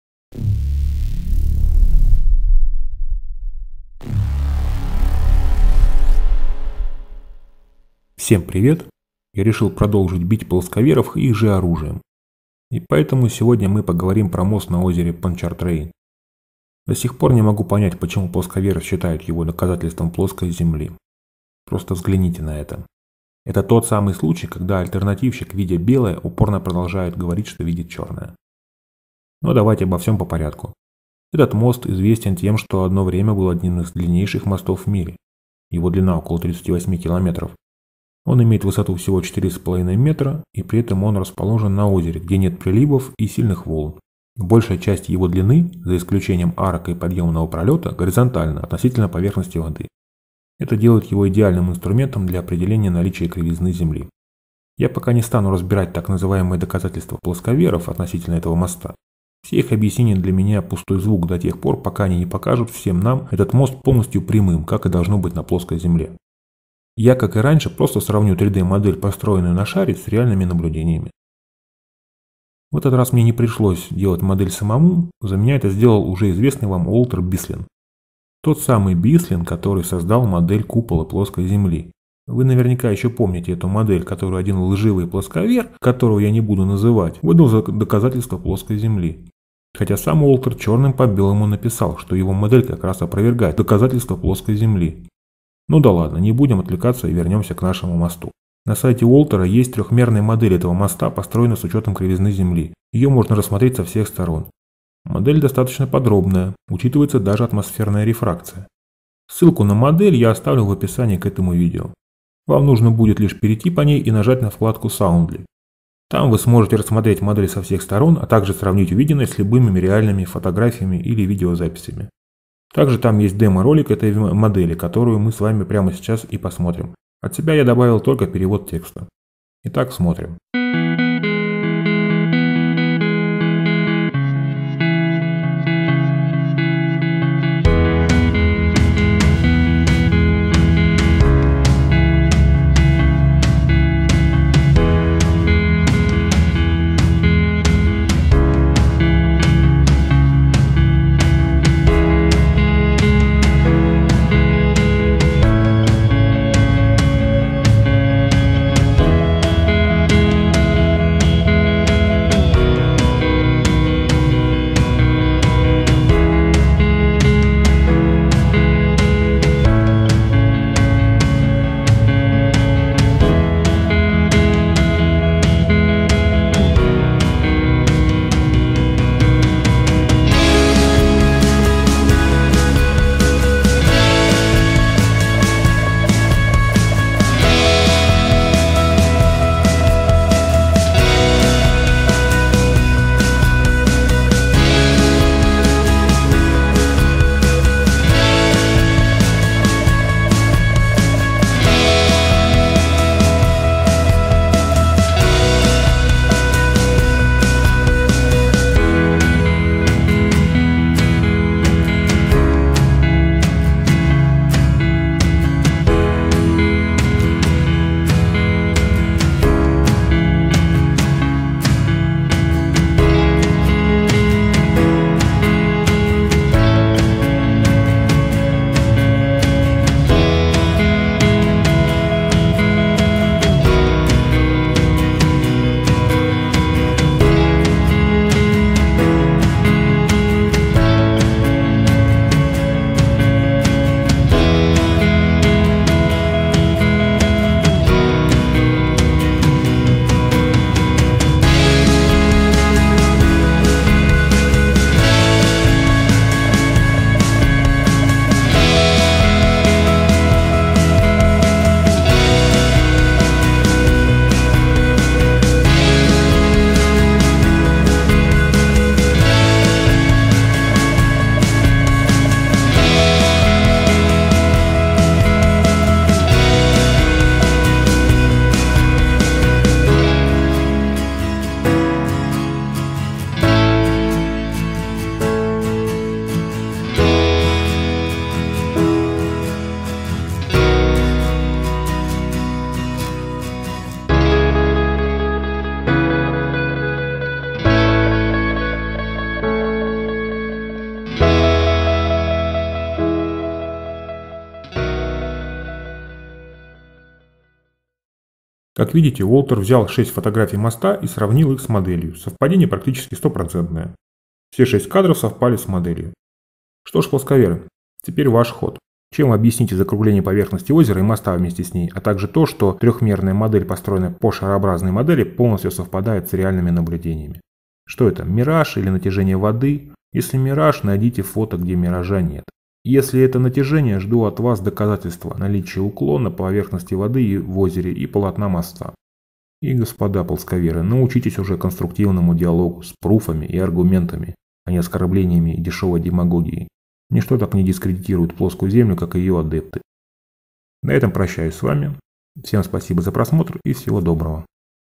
Плоская Земля. Как Пончартрейн натягивает плосковеров на глобус.
Продолжаем бить плосковеров их же оружием.